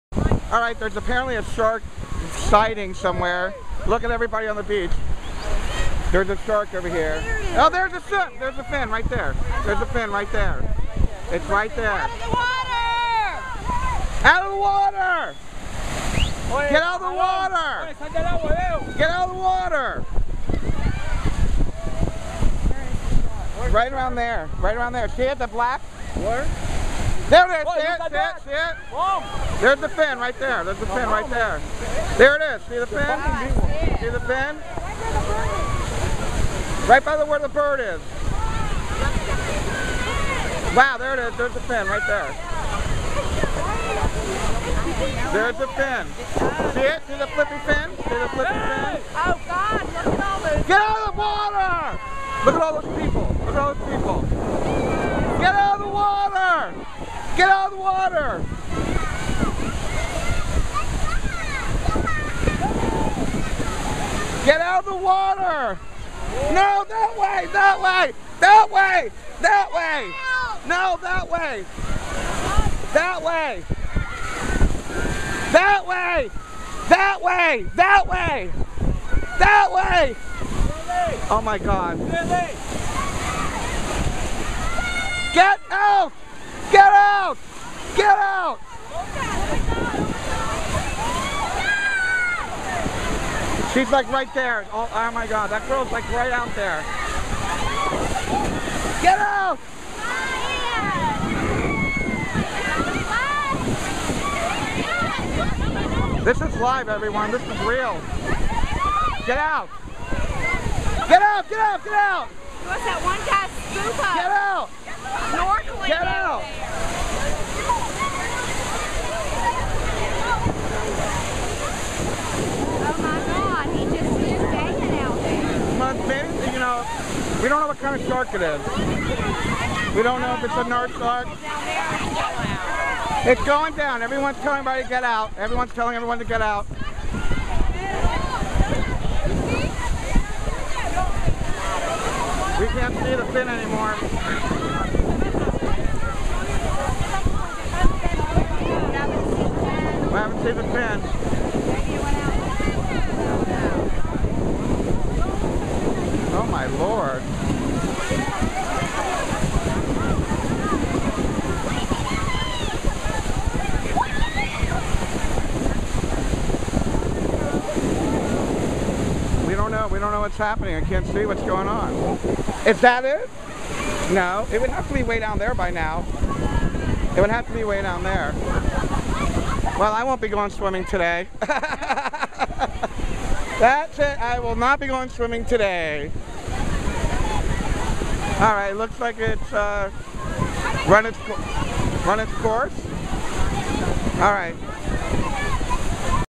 Shark Sighting Hollywood Beach 6-10-12
A bit of excitement on Hollywood Beach during my visit. Felt like a JAWS movie with everyone running on to the shore. I lost my voice from screaming so much...